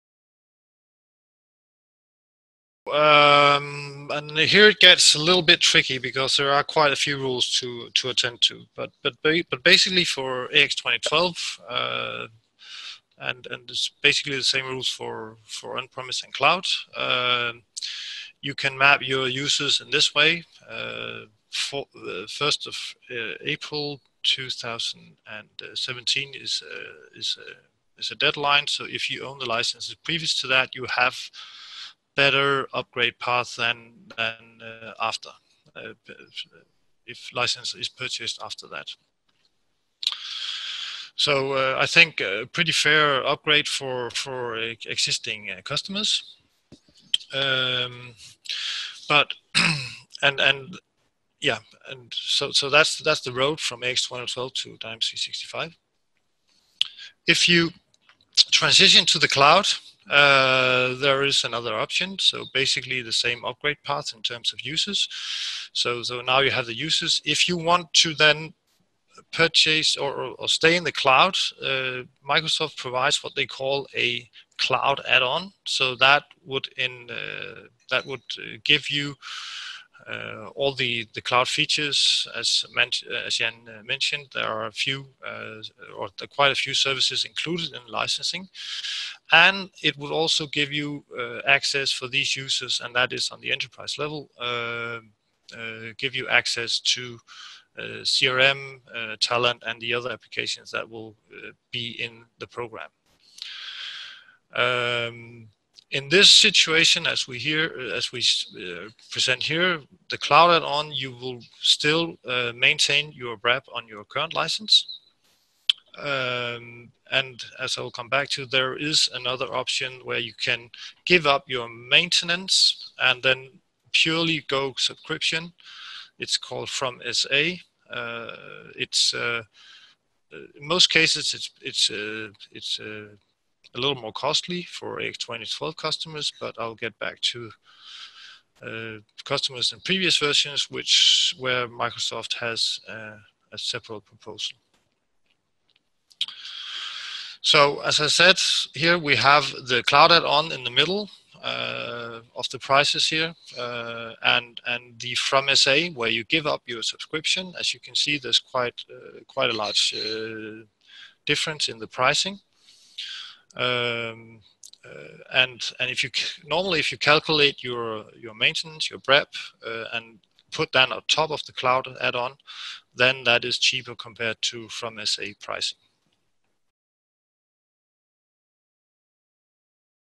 Upgrading from Microsoft Dynamics AX 2012 to Dynamics 365 explained
This webinar snippet describes the transition mapping for the AX2012 customers to Microsoft Dynamics 365.